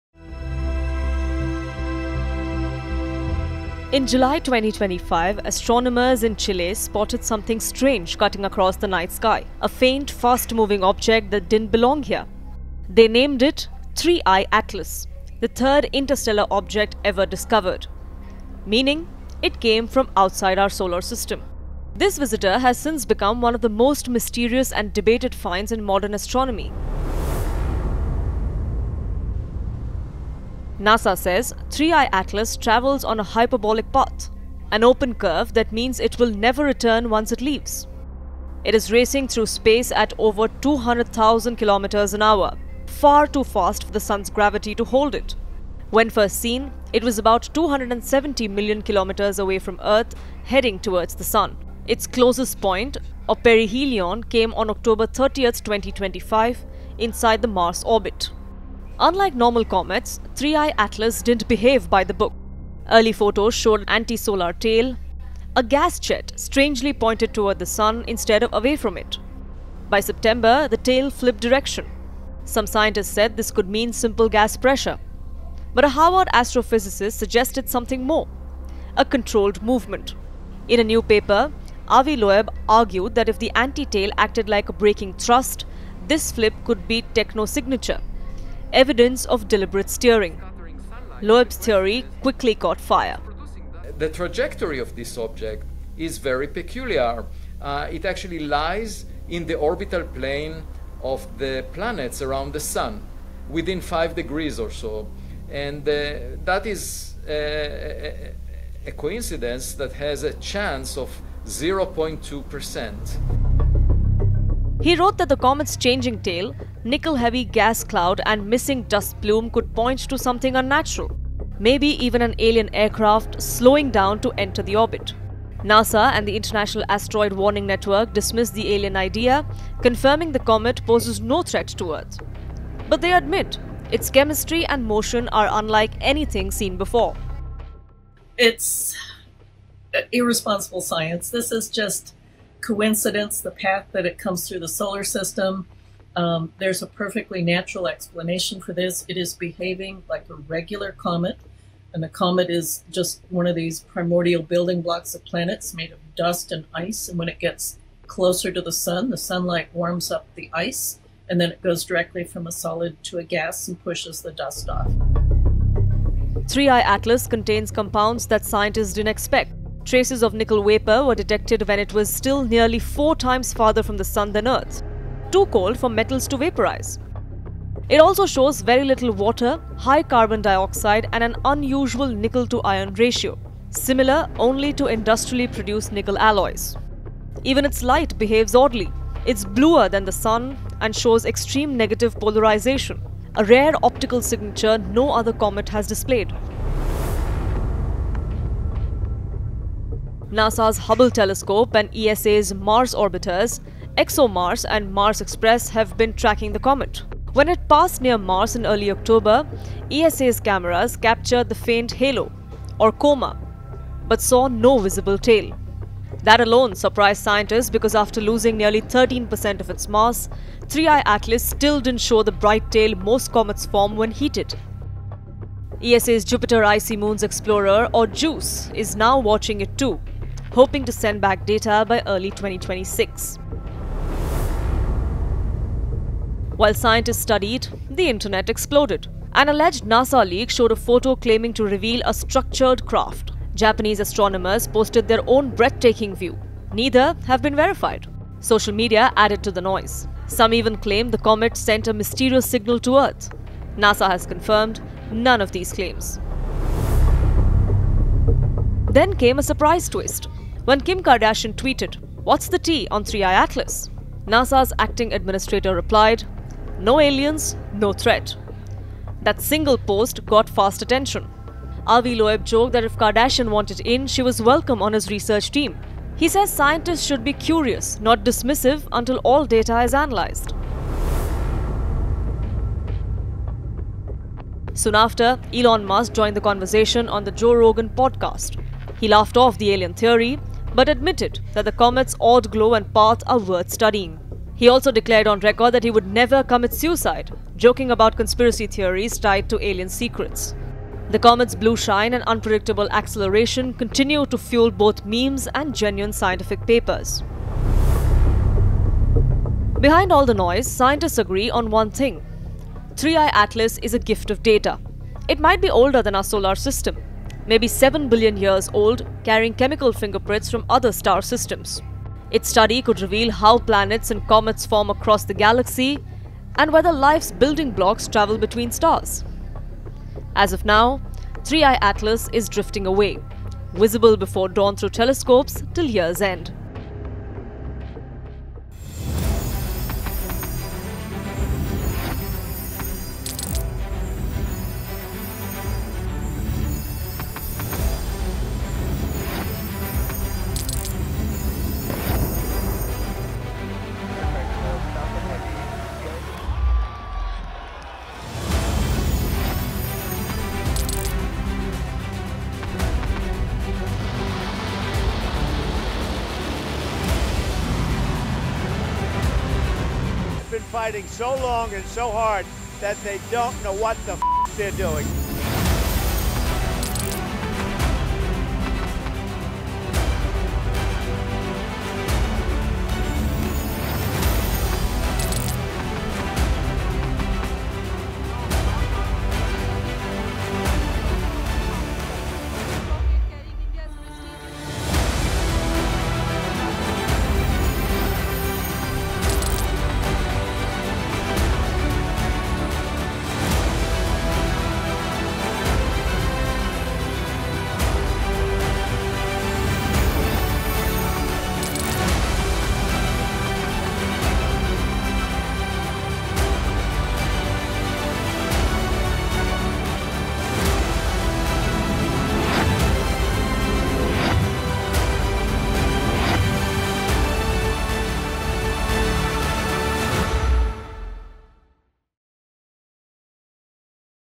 Why Is Everyone Talking About Harvard Astrophysicist Avi Loeb's 3I/ATLAS 'Alien' Theory? | NASA | 4K
In early July, astronomers spotted a mysterious object, later dubbed 3I/ATLAS after it was confirmed to be the third-ever interstellar visitor cruising through our solar system. Last week, the object, which is now generally believed to be a comet, reached its closest point to the Sun, or its perihelion, brightening up at an unexpected rate and turning “distinctly bluer.”
But Harvard astronomer Avi Loeb claims 3I/ATLAS could be an enormous alien spacecraft, pointing at numerous “anomalies” that make it a major outlier. The mystery surrounding 3I/ATLAS has captured the imagination of many, including A-listers like Kim Kardashian and tech mogul Elon Musk.

n18oc_crux  n18oc_world